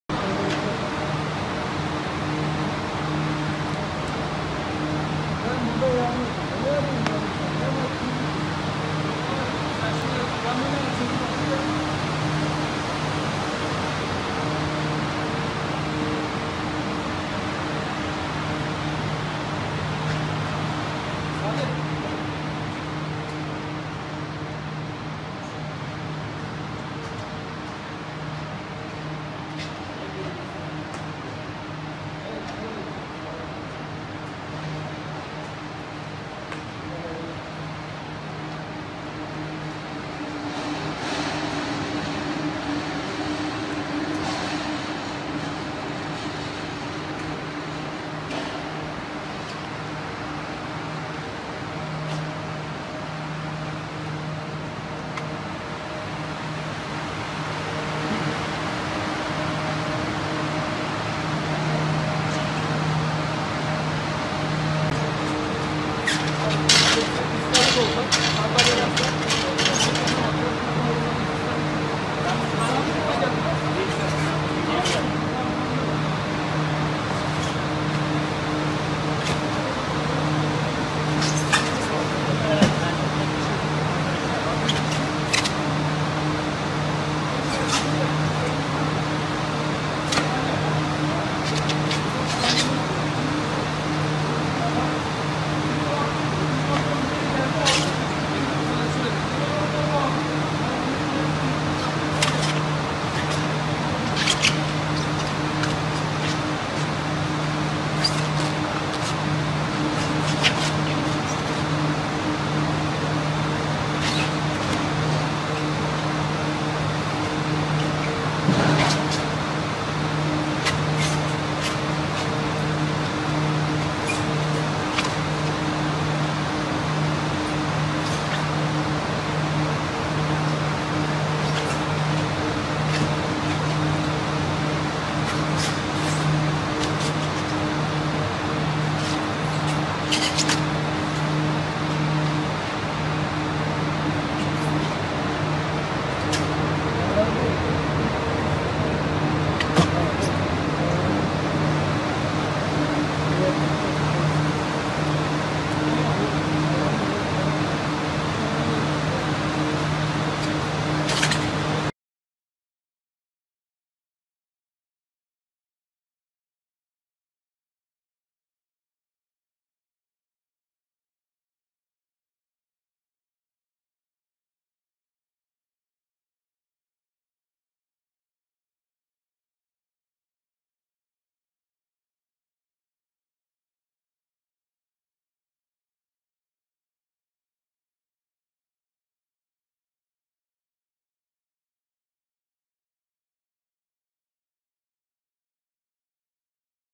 Yasak avlanan yarım ton balığına el konuldu
İstanbul Beylikdüzü’nde bulunan Gürpınar Su Ürünleri Hali’nde yasak dönemde avlanılan yarım ton ağırlığında ki  29 kutu istavrit balığına el konuldu.